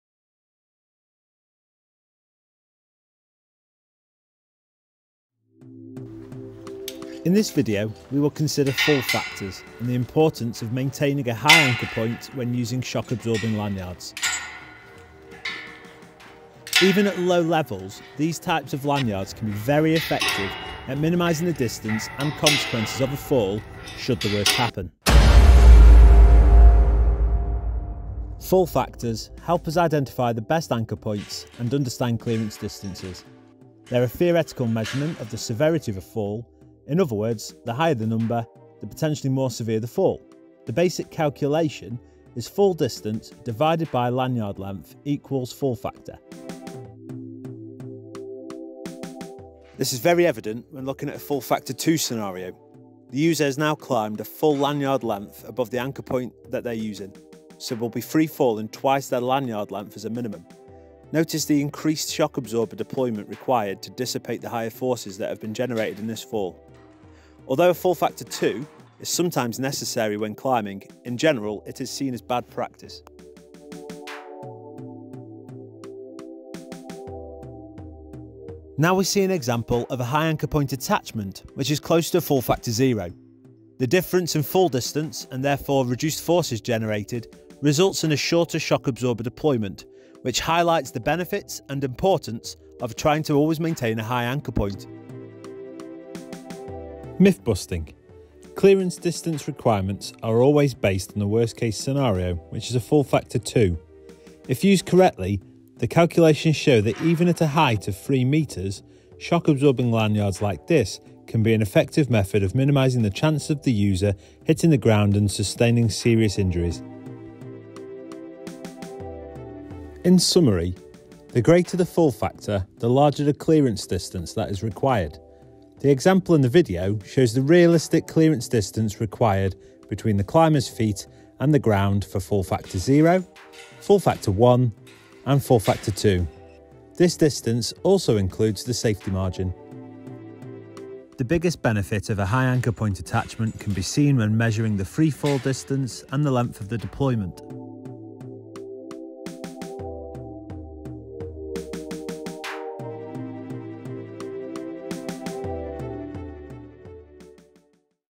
Personal Fall Protection Equipment - Fall Factors
This video tutorial focuses on Fall Factors when utilising typical Y-type shock absorbing fall arrest lanyards.

The impact forces generated in a fall are significant, and are dependant on user weight, fall distance and type of lanyard.
The 100kg 'dummy' is used to highlight how important the use of shock absorbing lanyards conforming to the correct standards are, but also emphasises the benefits of safe use and maintaining high anchor points when using any fall arrest equipment.